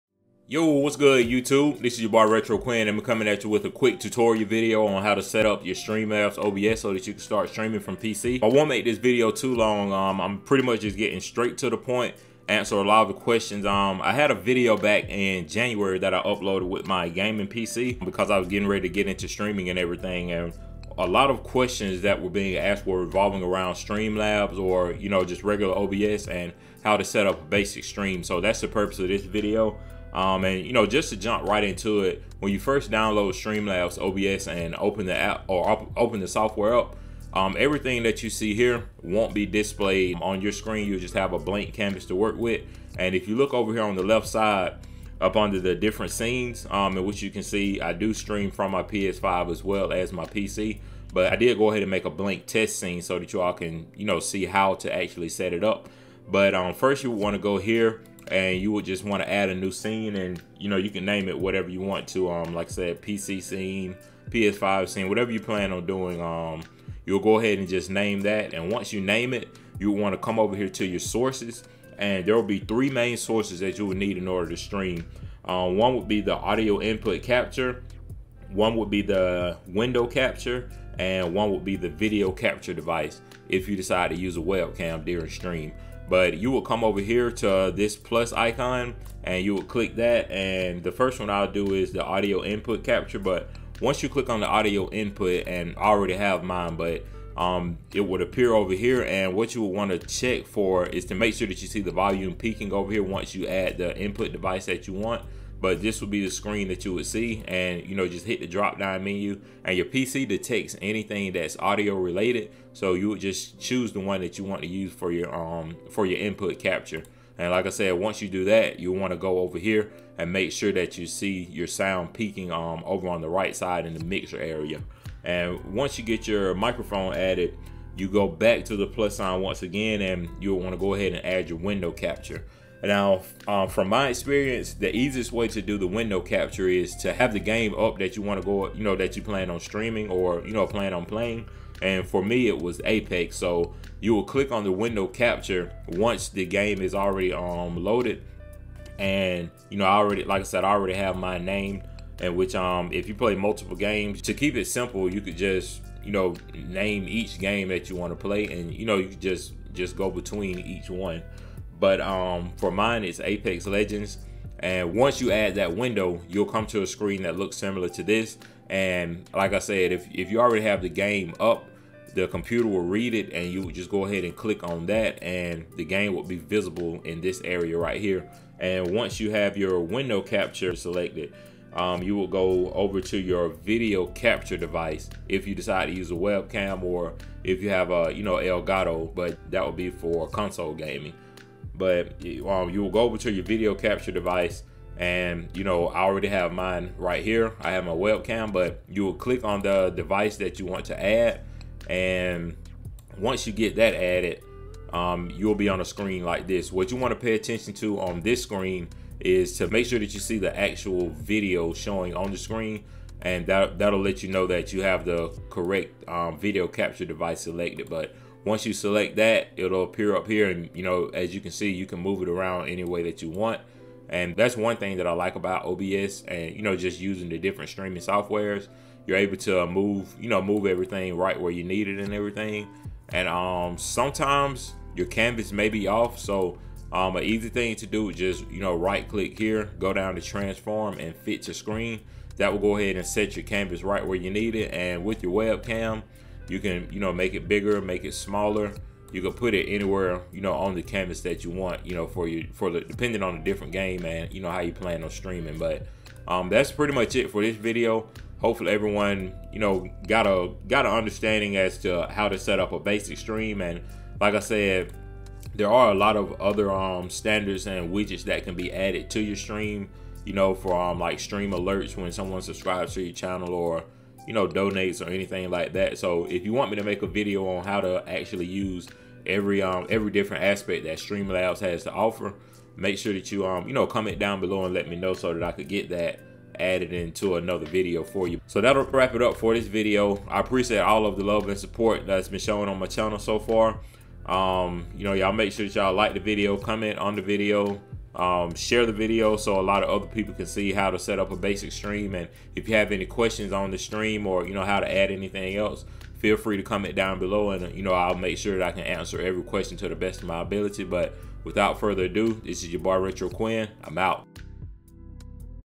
How To Stream With Streamlabs OBS (Updated 2021 Version) // Learn How To Stream In 5 minutes!!
Want to get into streaming from Console or PC, but you're not sure how to get started with Streamlabs OBS? Look no further because in less than 5 minutes you can learn how to set up a quality stream.  In this video I will be showing you how to add a audio input device, webcam, and display capture for your gaming content.  As always, I appreciate the continued love and support and I look forward to interacting with everyone! *I had to reupload because the last file was corrupted.*